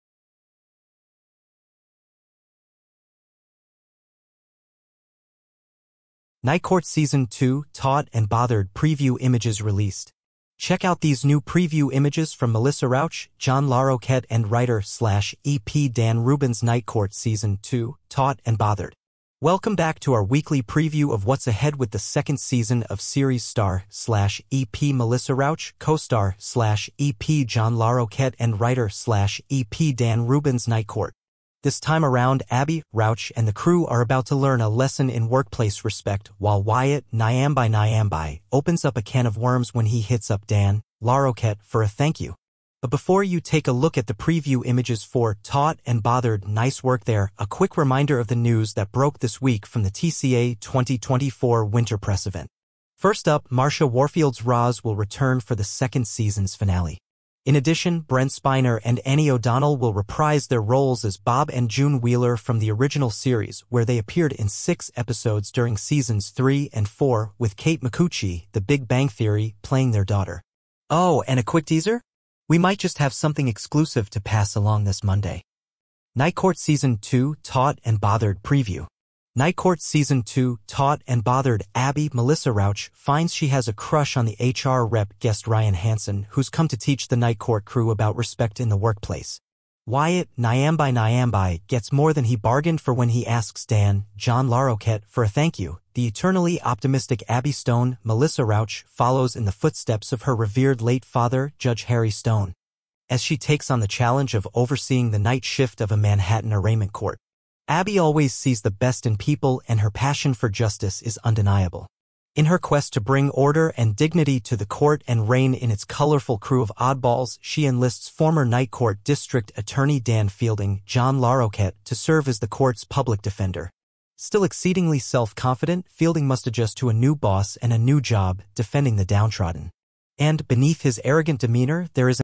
Night Court Season 2 "Taught and Bothered" Preview Images Released
Check out these new preview images from Melissa Rauch, John Larroquette & writer/EP Dan Rubin's Night Court Season 2, "Taught and Bothered."
Welcome back to our weekly preview of what's ahead with the second season of series star/EP Melissa Rauch, co-star/EP John Larroquette & writer/EP Dan Rubin's Night Court. This time around, Abby (Rauch) and the crew are about to learn a lesson in workplace respect, while Wyatt (Nyambi Nyambi) opens up a can of worms when he hits up Dan (Larroquette) for a "thank you." But before you take a look at the preview images for "Taught and Bothered" (nice work there), a quick reminder of the news that broke this week from the TCA 2024 Winter Press Event. First up, Marsha Warfield's Roz will return for the second season's finale. In addition, Brent Spiner & Annie O'Donnell will reprise their roles as Bob & June Wheeler from the original series (where they appeared in six episodes during Seasons 3 & 4) – with Kate Micucci (The Big Bang Theory) playing their daughter. Oh, and a quick teaser? We might just have something exclusive to pass along this Monday…
Night Court Season 2 "Taught and Bothered": Abby (Melissa Rauch) finds she has a crush on the HR rep (guest Ryan Hansen) who's come to teach the Night Court crew about respect in the workplace. Wyatt (Nyambi Nyambi) gets more than he bargained for when he asks Dan (John Larroquette) for a thank you.The eternally optimistic Abby Stone (Melissa Rauch) follows in the footsteps of her revered late father, Judge Harry Stone, as she takes on the challenge of overseeing the night shift of a Manhattan arraignment court.

Abby always sees the best in people, and her passion for justice is undeniable. In her quest to bring order and dignity to the court and reign in its colorful crew of oddballs, she enlists former night court district attorney Dan Fielding (John Larroquette) to serve as the court's public defender. Still exceedingly self-confident, Fielding must adjust to a new boss and a new job – defending the downtrodden. And, beneath his arrogant demeanor, there is an empathetic side to Fielding that Abby is determined to unearth.